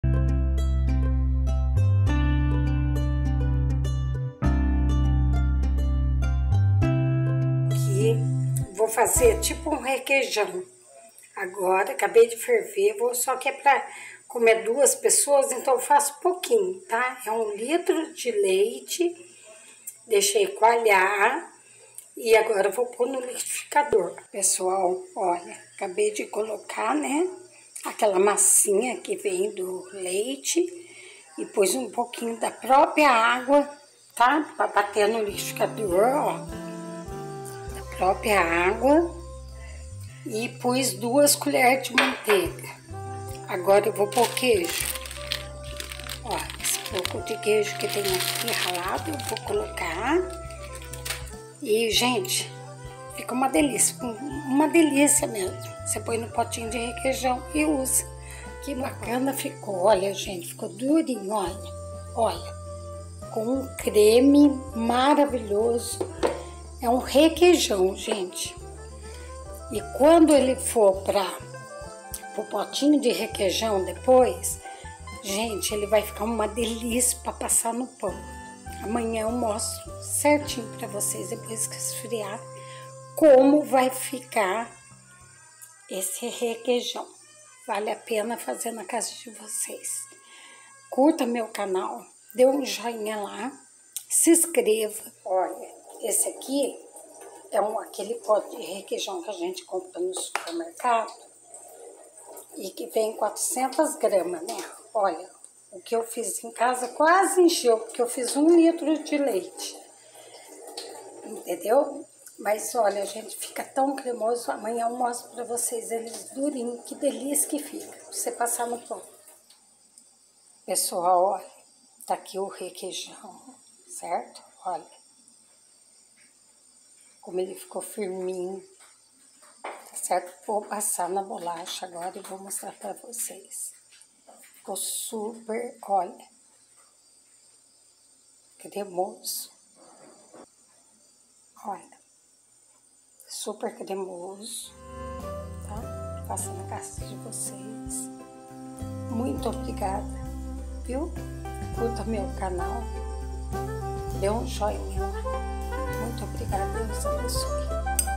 REQUEIJÃO CASEIRO
RECEITA:

1 Litro de leite
3 colheres de suco de limão ou vinagre (4)
1 pitada de sal
Deixar qualhar
Vai para a peneira e espremer até sai toda liquido
Amassar bem com a colher e levar ao Liquidificador
2 colheres de sopa de manteiga com sal
Um pouco do liquido penerado
50 gramas de queijo ralado parmesão
Bater durante dois (2) minutos.
Colocar no pote

NEUSA TOLEDO